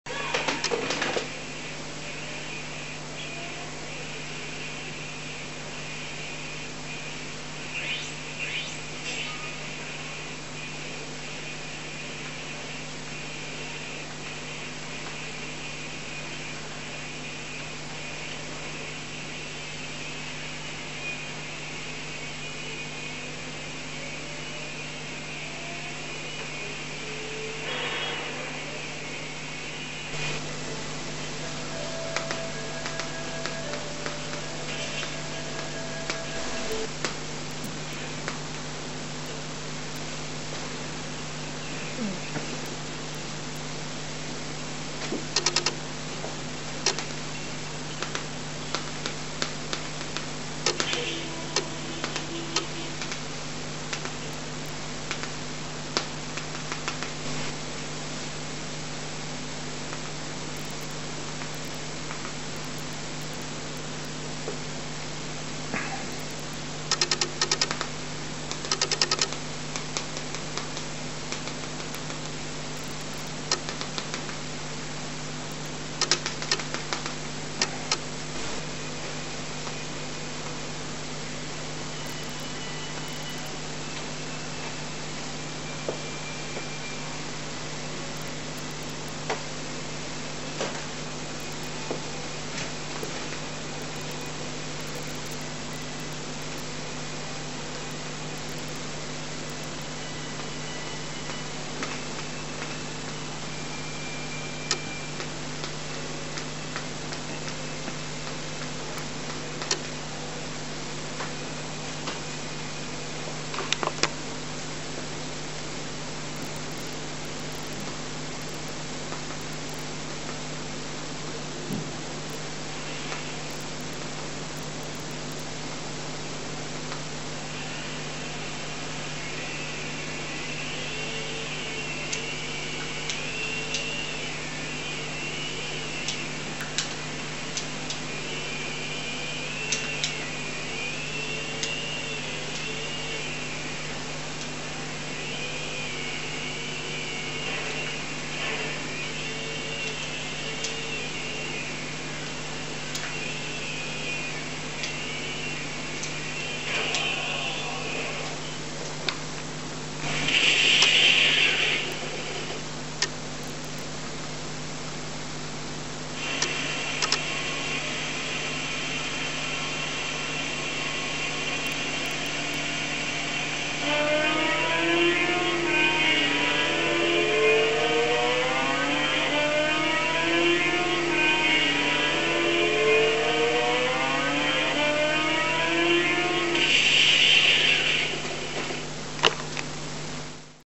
YINLIPS YDPG18 Android Games HDMI Test
Android Tablet/Console Yinlips YDPG18
Demo de algunos juegos android ejecutando con la salida HDMI.
He puesto el volumen de la consola al mínimo, para que se pueda escuchar cuando presiono los botones y hago click en la pantalla táctil, que funciona como un Touchpad cuando está la pantalla conectada.
Running some android games using HDMI Output.
I left the console volume to a minimum to hear when I press the keys and I click on the touch screen, which functions as a touchpad when an external display is connected through HDMI.